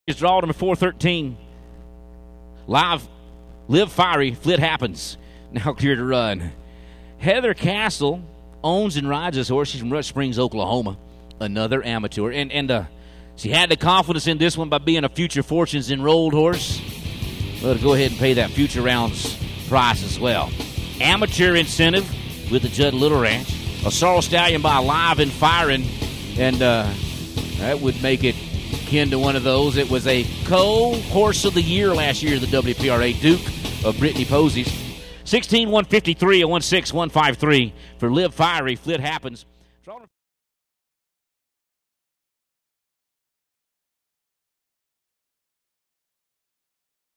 Heather Castle BFA 2012
Heather Castle running at the BFA 2012.  Video by visworks.com